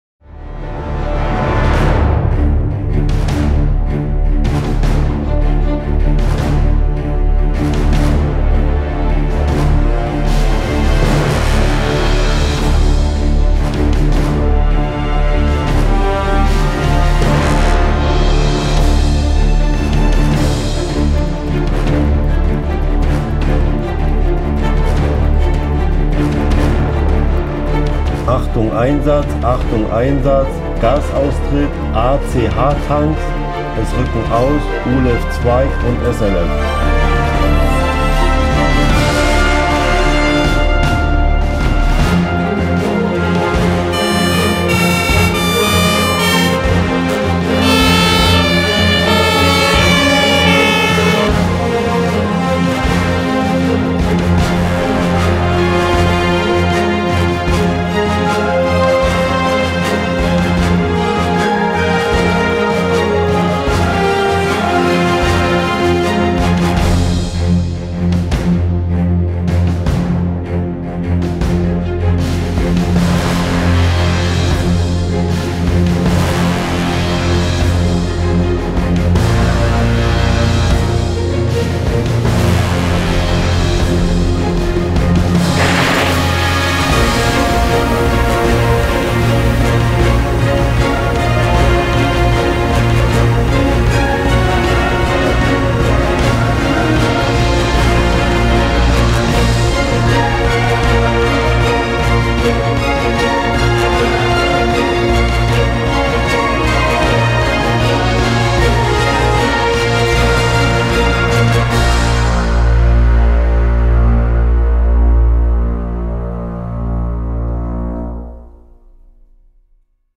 scanfeld - Gas release alarm!
It’s always easier to explain with pictures what you actually do. So, we made a movie!
This short video illustrates how our scanfeld units monitor an industrial site. When an unexpected gas release is detected, an alarm is generated in the control room. The scanfeld user interface displays the position, type, and extension of the gas cloud. The situation is assessed and a decision to act is taken. Here, the fire brigade deploys in order to mitigate the situation by a rapid water screen.
Alarm - Assessment - Action - De-escalation

Stop guessing. Start Knowing.